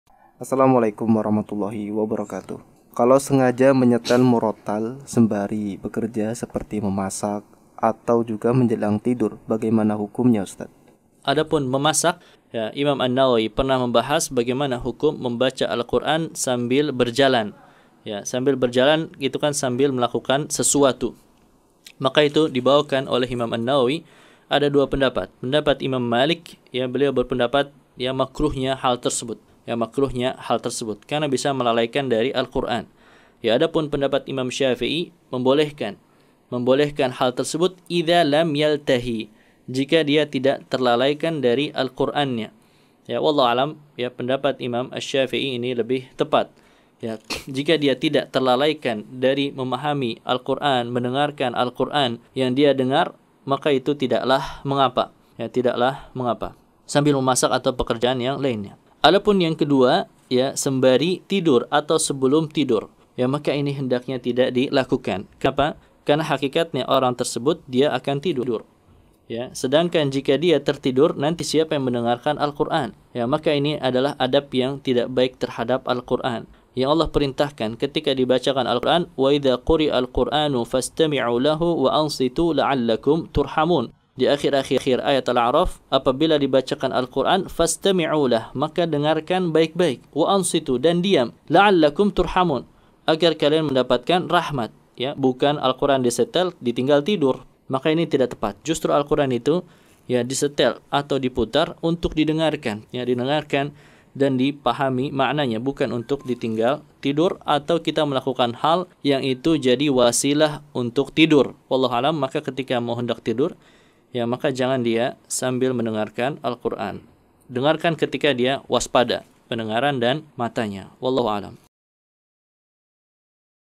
Hukum mendengar Al-Qur'an sambil beraktivitas atau tidur ( Sesi Tanya Jawab : Ismail Radio )
Pertanyaan :
Bagaimana hukum mendengarkan Al-Qur'an sambil aktivitas atau tidur ?

Prmateri :
🔊 Ust. Varian Ghani Hirma, Lc

Diambil dari Tanya Jawab Kajian Kitab At-Tibyan fi Adabi Hamalatil Qur'an. Di Radio Ismail AM 774 Khz